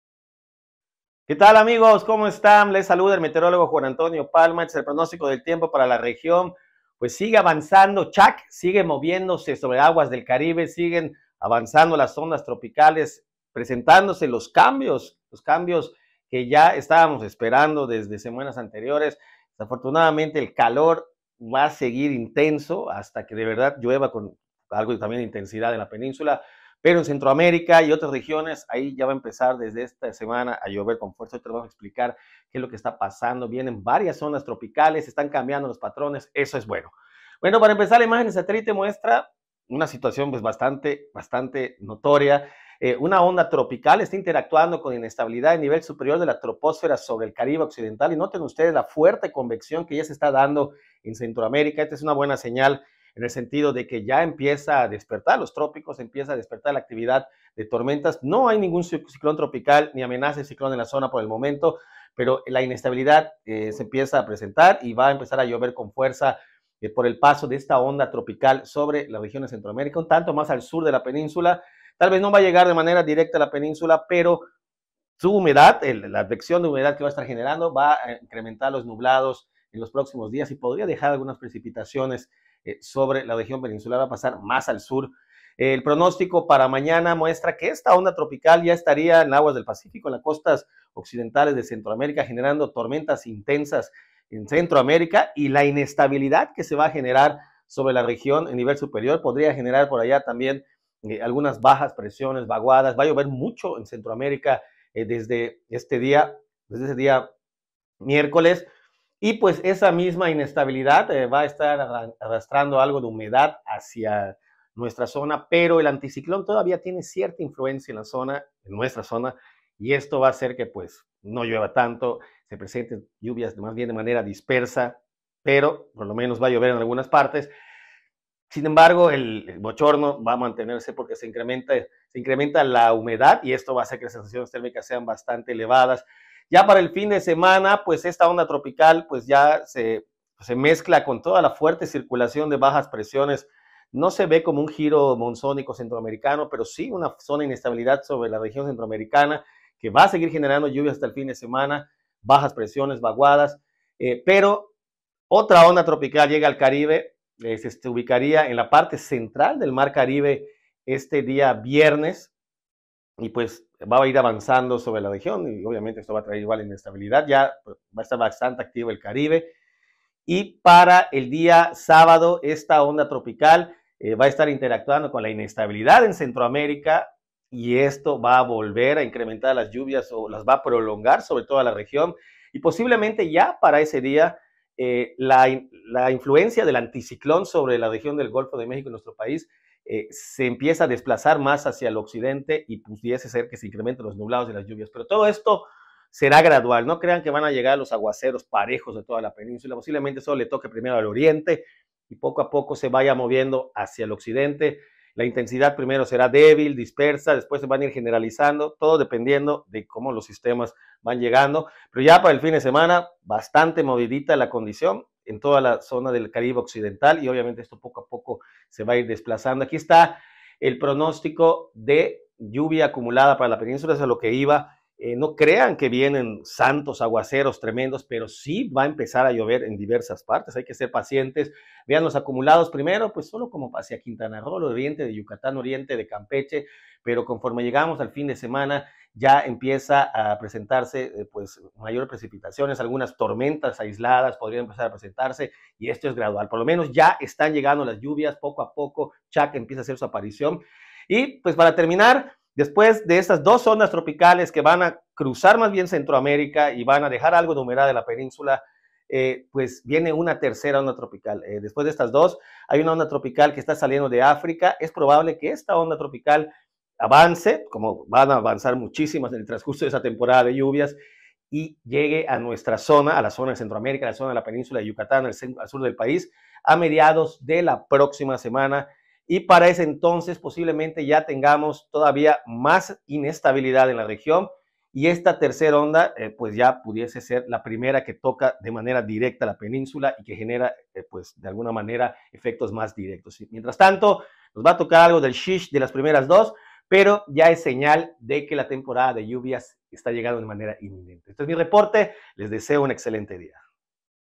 ⛈️ Primera onda tropical de la temporada llegará en breve a la región
Otra más se aproximaría el fin de semana.